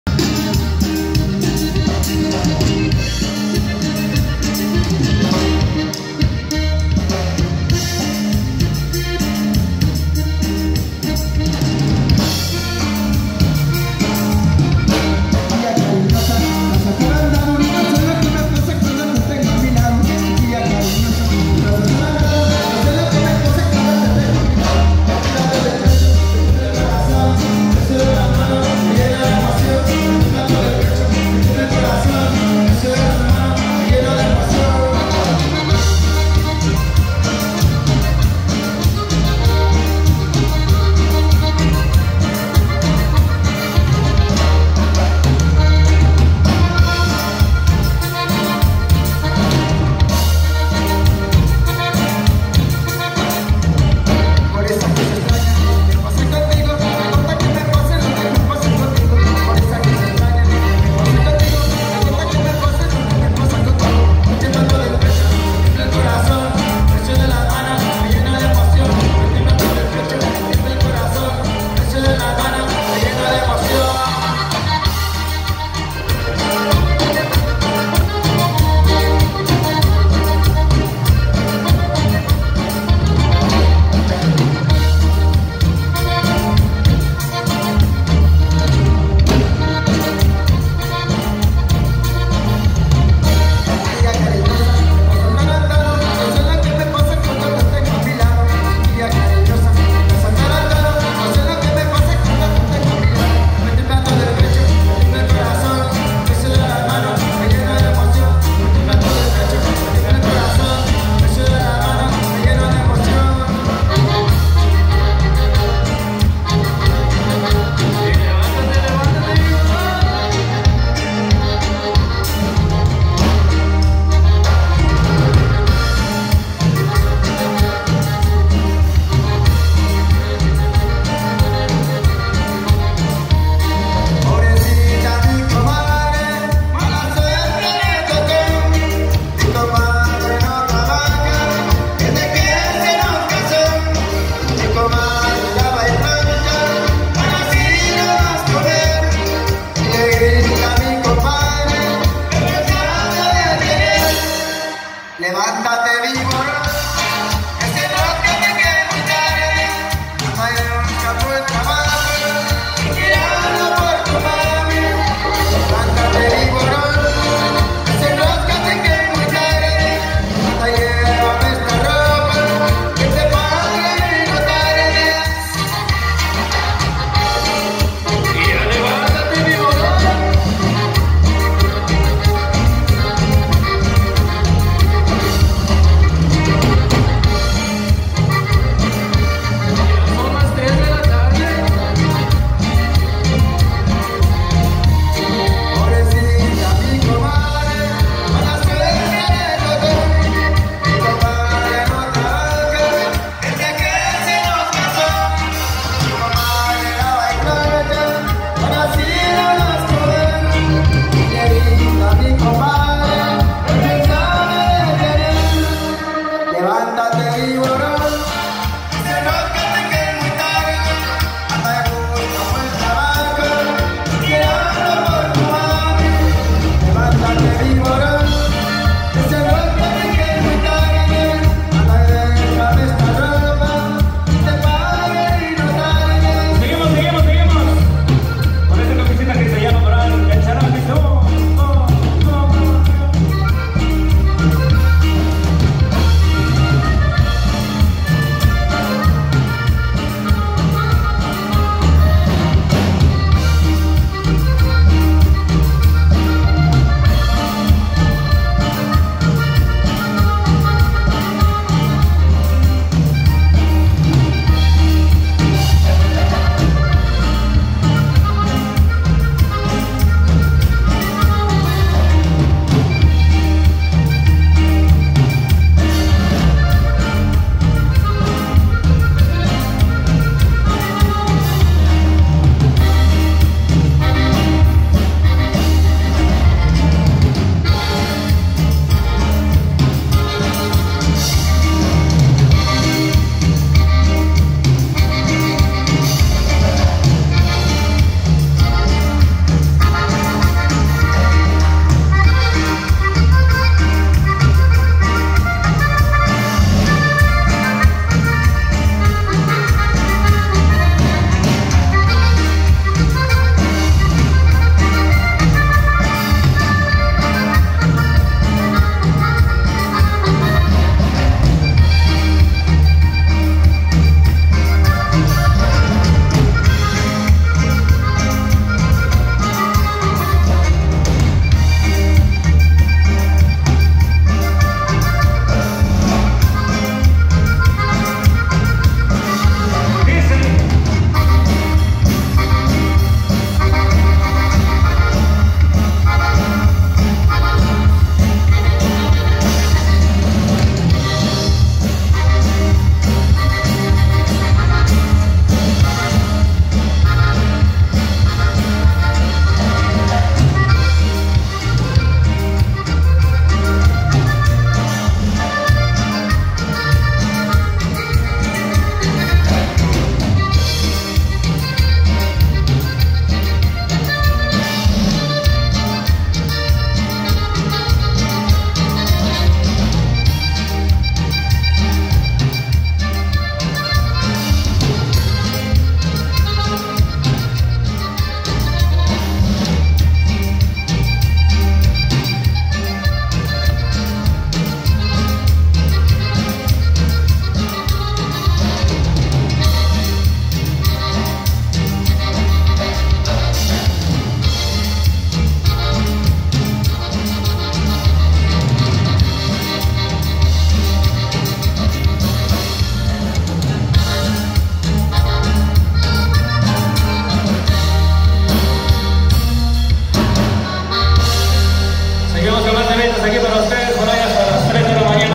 Popurri De Cumbias con Ricardo y Genaro-Grupo En Vivo
Un popurri de cumbias en vivo desde Houston, Texas con Ricardo y Genaro.